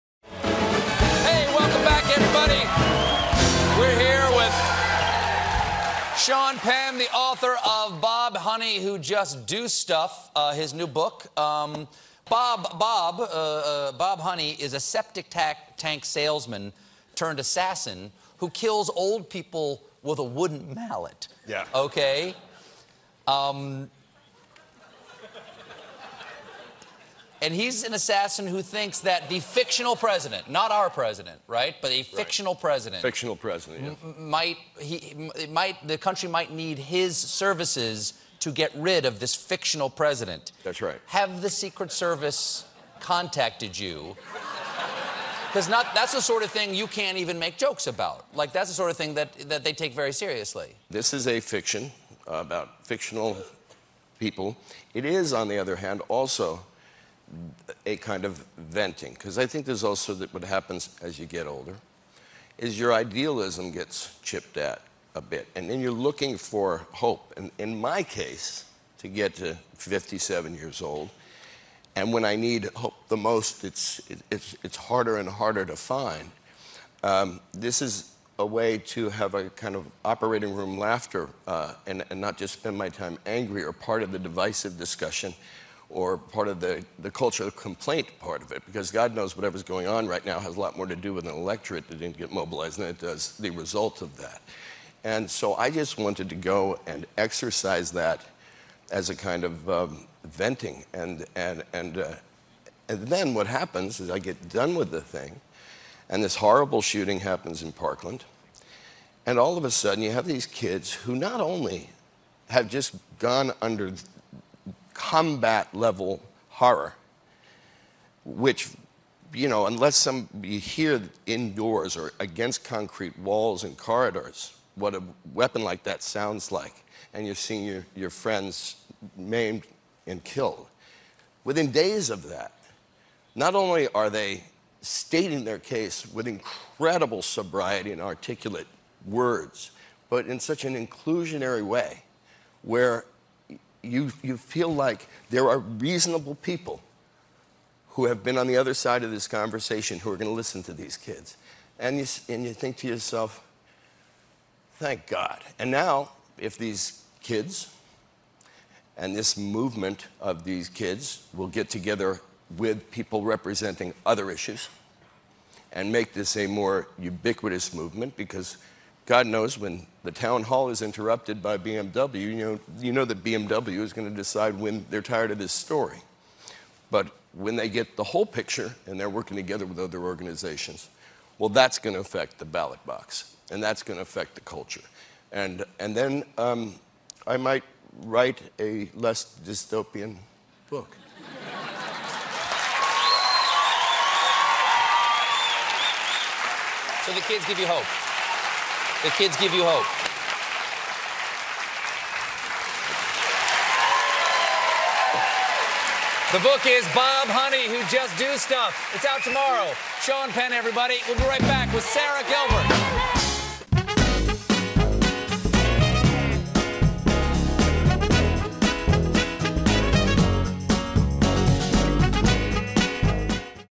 Sean Penn Explains Why His Novel Is A Dystopian
'Bob Honey Who Just Do Stuff: A Novel' author Sean Penn describes how the current state of affairs are reflected in his new book.